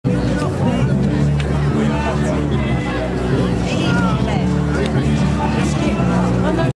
Se non ora quando?
Mobilitazione nazionale delle donna a Imperia.
un successone, mille persone e passa!